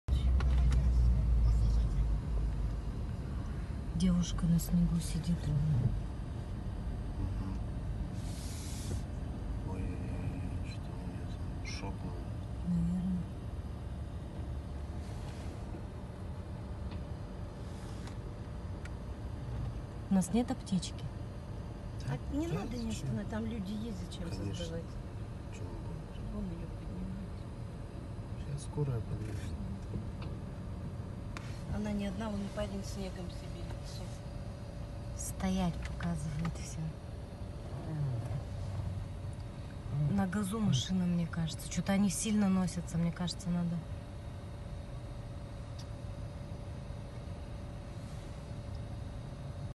ДТП на пересечении улиц Жамбыла и Абая
24 ноября около 11.20 минут на пересечении улиц Жамбыла и Абая произошло столкновение пожарной автомашины с автомобилем Volkswagen. Установлено, что водитель спецтранспорта двигался со специальным сигналом. По факту ДТП собран материал по 610 статье КРКоАП.